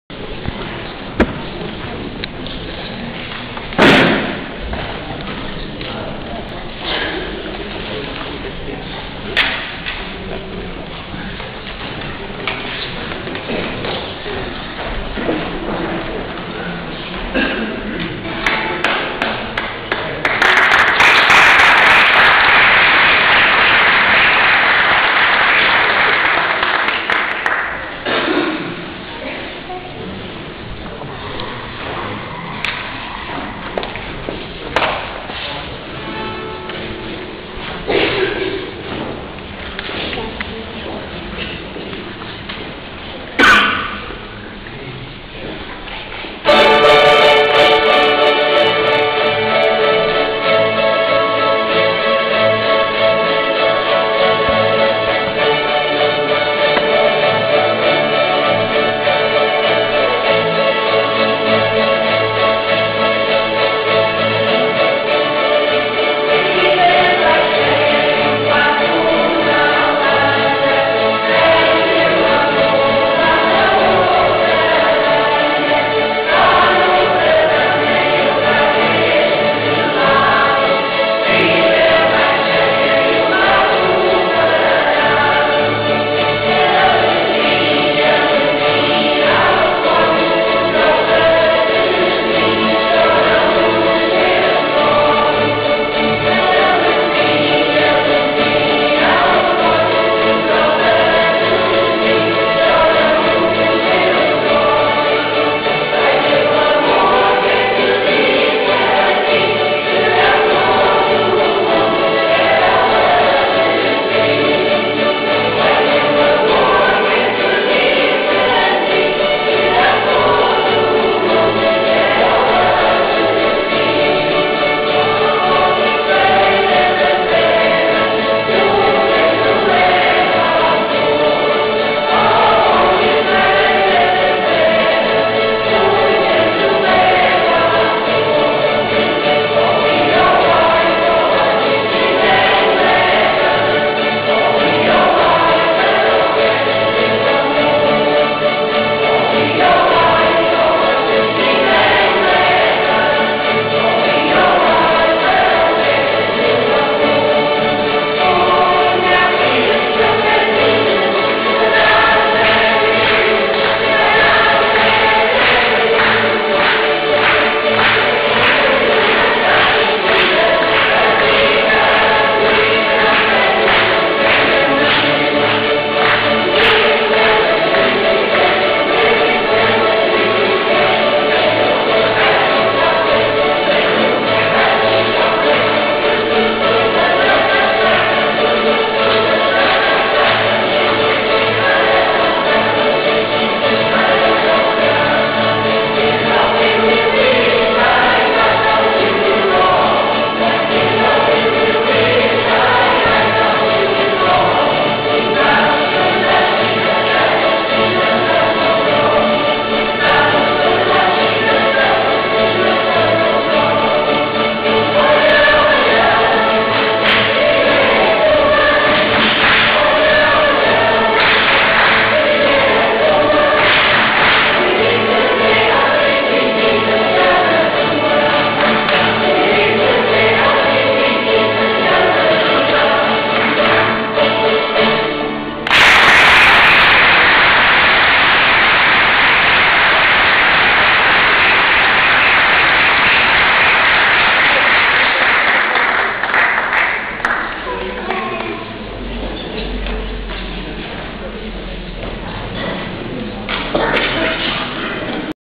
S.F.I.A. 2011 - CAVAQUINHOS - CAFÉ CONCERTO
Participação do Grupo de Cavaquinhos da SFIA no Café-Concerto, dia 3 de Dezembro de 2011, no Salão de Festas, com uma rapsódia de música popular portuguesa.